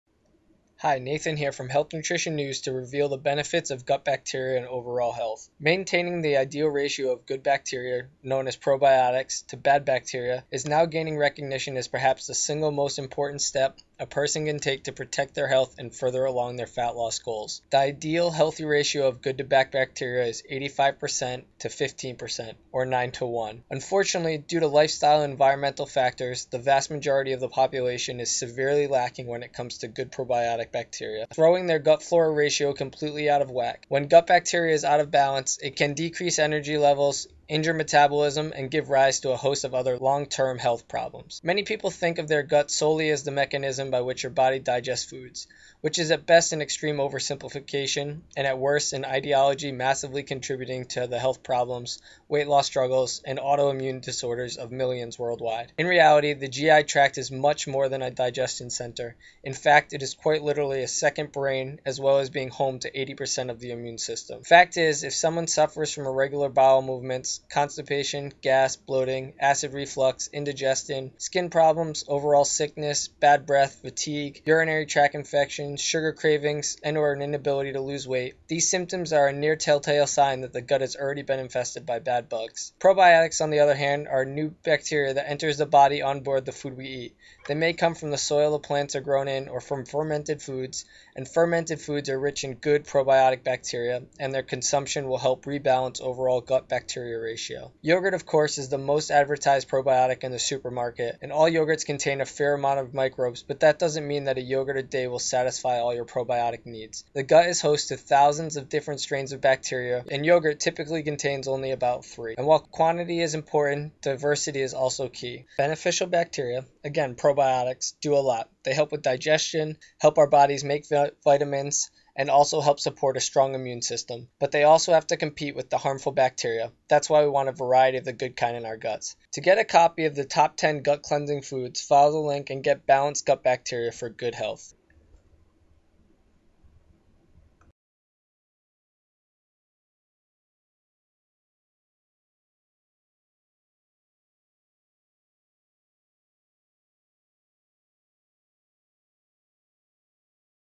Balanced Gut Bacteria Linked to Good Health
Maintaining the ideal ratio of "good bacteria" (known as probiotics) to "bad bacteria" is now gaining recognition as perhaps the single most important step a person can take to protect their health and further along their fat loss goals. The ideal healthy ratio of "good" to "bad" bacteria is 85% to 15%, or 9 to 1.

Unfortunately, due to lifestyle and environmental factors, the vast majority of the population is severely lacking when it comes to good probiotic bacteria, throwing their gut flora ratio completely out of whack. When gut bacteria is out of balance, it can decrease energy levels, injure metabolism, and give rise to a host of other long-term health problems.

Many people think of their gut solely as the mechanism by which your body digests food, which is at best an extreme oversimplification, and at worst an ideology massively contributing to the health problems, weight loss struggles, and auto-immune disorders of millions world-wide.
In reality, the G.I. tract is MUCH more than a digestion center; in fact, it is quite literally a second brain as well as being "home" to 80% of the immune system

Fact is, if someone suffers from irregular bowel movements, constipation, gas, bloating, acid reflux, indigestion, skin problems, overall sickness, bad breath, fatigue, urinary tract infections, sugar cravings, and/or an inability to lose weight, these symptoms are a near telltale sign that the gut has ALREADY been infested by "bad" bugs.

Probiotics, on the other hand, are new bacteria that enters the body onboard the food we eat. They may come from the soil the plants are grown in, or from fermented foods and Fermented foods are rich in "good" probiotic bacteria, and their consumption will help rebalance overall gut bacteria ratio.

Yogurt, of course, is the most advertised probiotic in the supermarket. All yogurts contain a fair amount of microbes but that doesn't mean that a yogurt a day will satisfy all your probiotic needs. The gut is host to thousands of different strains of bacteria (yogurt typically contains three), and while quantity is important, diversity is also key.
Beneficial bacteria (Probiotics) do a lot for they help with digestion, help our bodies make vitamins, and also help support a strong immune system. But they also have to compete with the harmful bacteria. That's why we want a variety of the good kind in our guts.
To get a free copy of the Top 10 Gut Cleansing Foods follow the link and get balanced gut bacteria for good health.